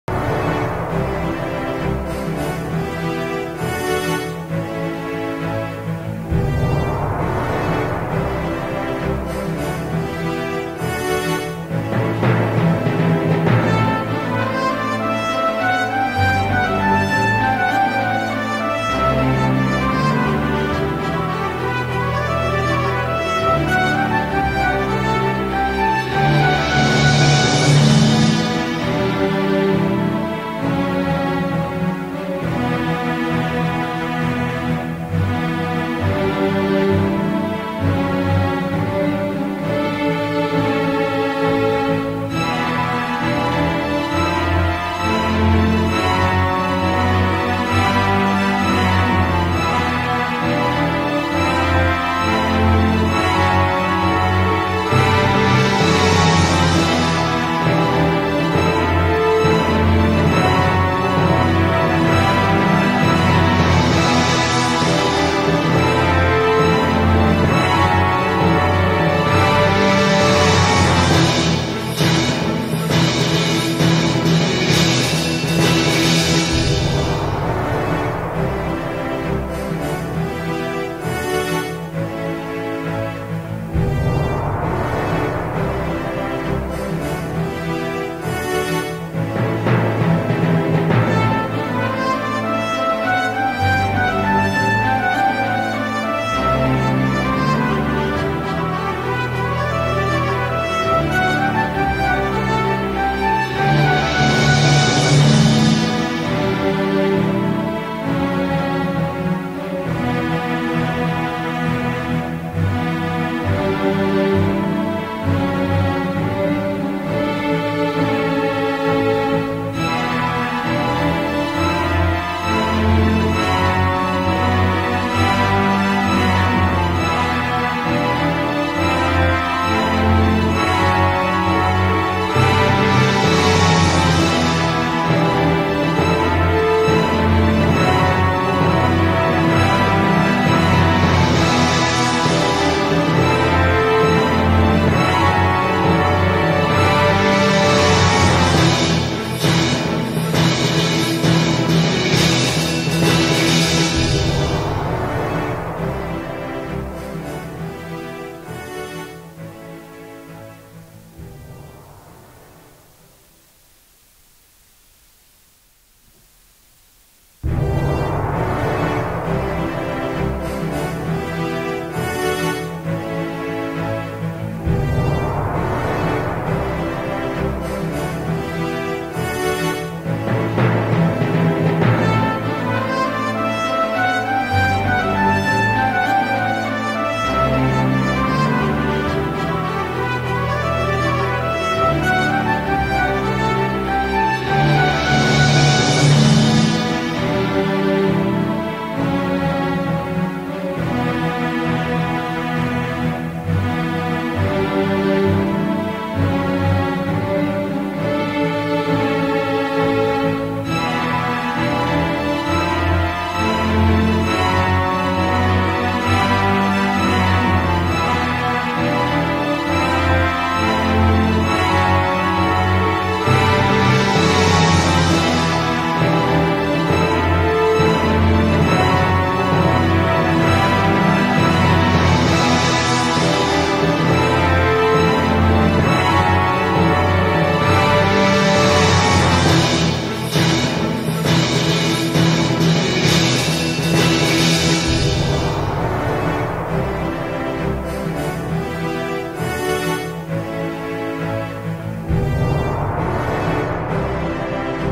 Тренировка на "Берое" в Белек
Тренировки на "Берое" в Турция - 18 януари 2019 г.